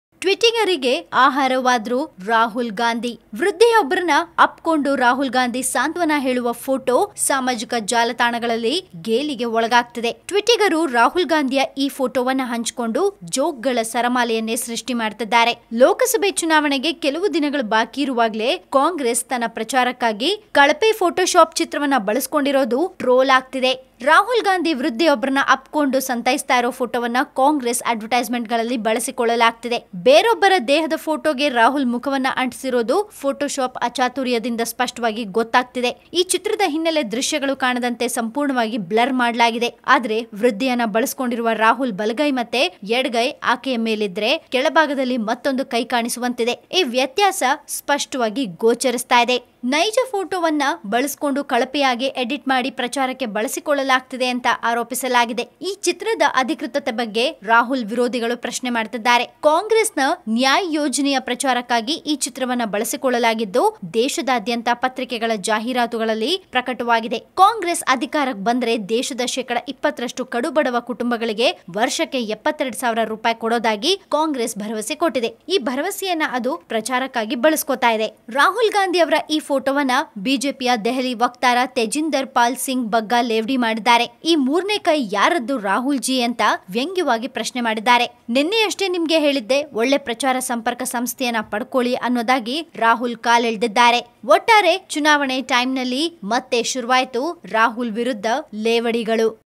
ಮತ್ತೆ ಟ್ವಿಟ್ಟಿಗರ ಟ್ರೋಲ್ ಗೆ ಆಹಾರವಾದ ರಾಹುಲ್ ಗಾಂಧಿ
Congress President Rahul Gandhi gets trolled on Social Media for photo shopped image used in Nyay scheme advertisements.
ವೃದ್ಧೆಯೊಬ್ಬರನ್ನು ಅಪ್ಪಿಕೊಂಡು ರಾಹುಲ್ ಗಾಂಧಿ ಸಾಂತ್ವನ ಹೇಳುವ ಚಿತ್ರ ಸಾಮಾಜಿಕ ಜಾಲತಾಣಗಳಲ್ಲಿ ಗೇಲಿಗೊಳಗಾಗುತ್ತಿದೆ. ಟ್ವಿಟ್ಟಿಗರು ರಾಹುಲ್ ಗಾಂಧಿಯ ಈ ಚಿತ್ರವನ್ನು ಹಂಚಿಕೊಂಡು ಜೋಕ್‌ಗಳ ಸರಮಾಲೆಯನ್ನೇ ಸೃಷ್ಟಿಸುತ್ತಿದ್ದಾರೆ.